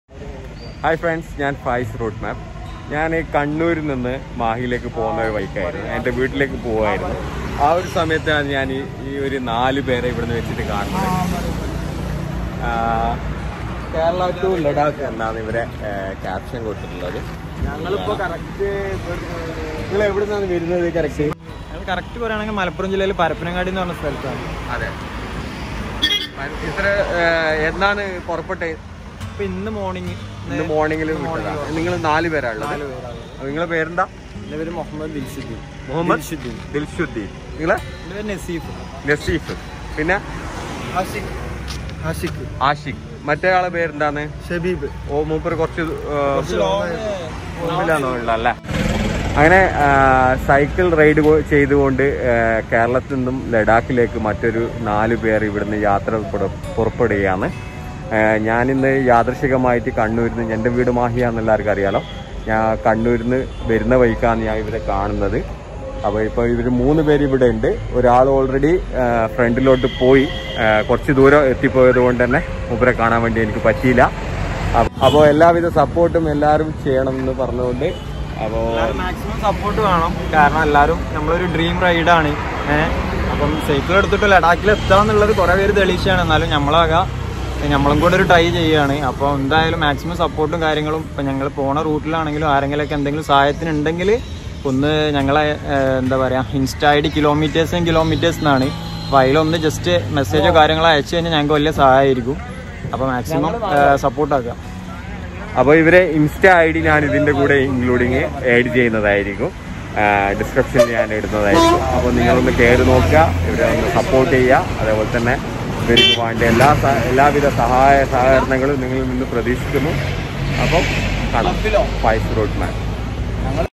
Faiz Route Map കേരളത്തിൽ നിന്നും ലഡാക്കിലേക്ക് സൈക്കിളിൽ യാത്രതിരിച്ച നാല് ചെറുപ്പക്കാർ
Faiz Route Map Vlog | You Can Watch Nice travel videos 😊

Instagram  name  kilometers-kilometers